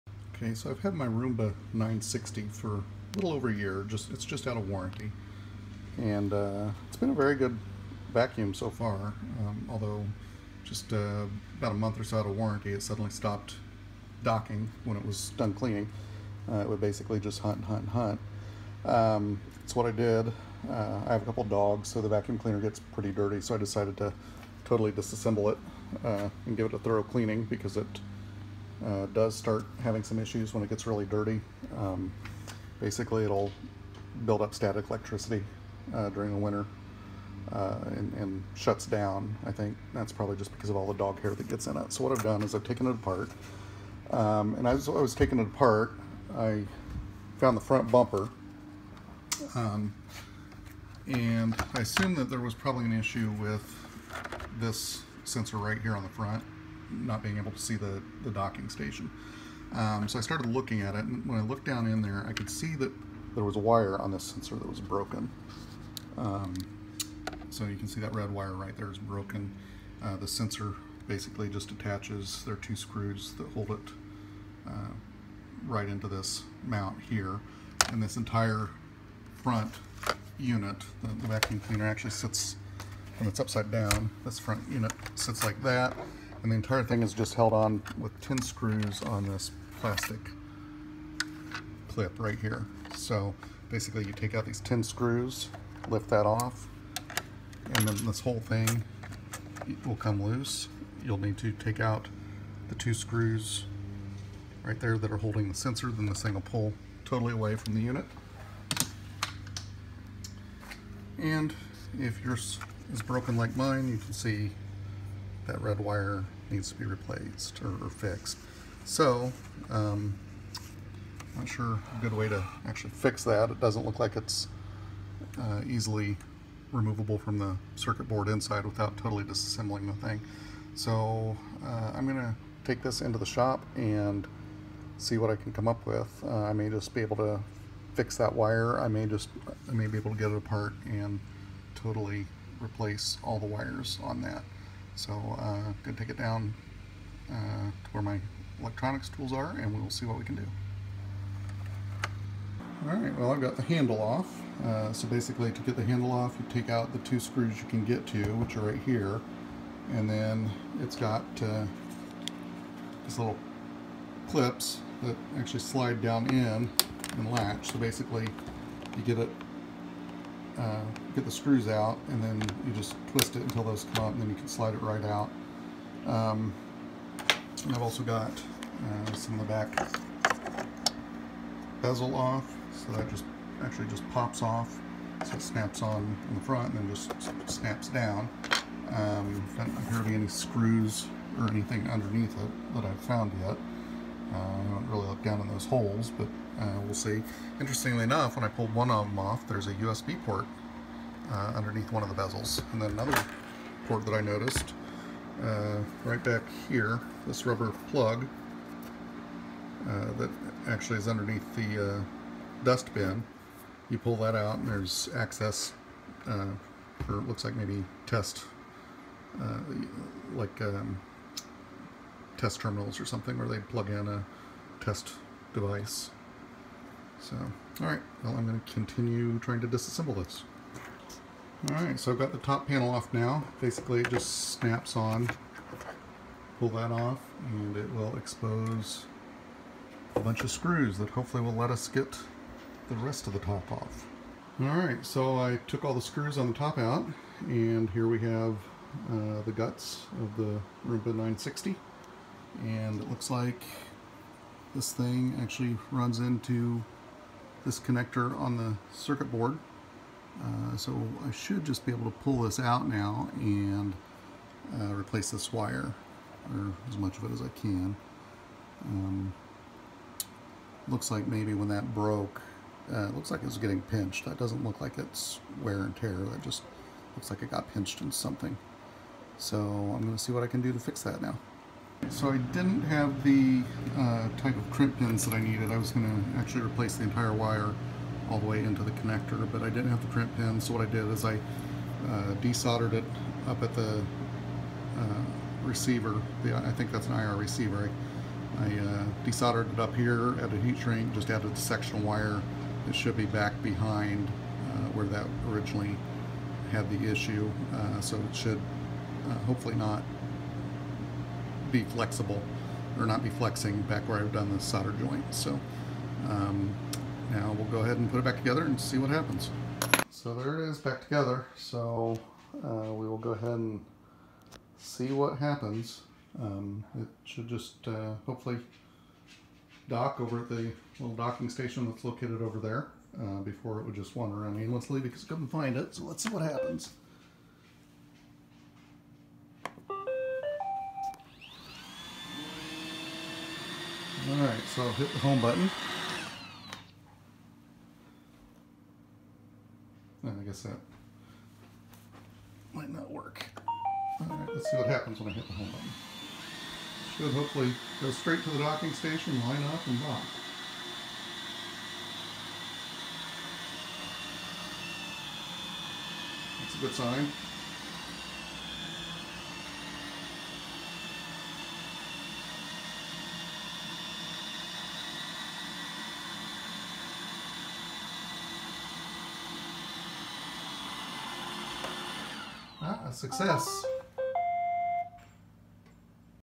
Roomba 960 Docking Fix
Teardown and repair of a bad docking sensor on a Roomba 960 that can't find the base station.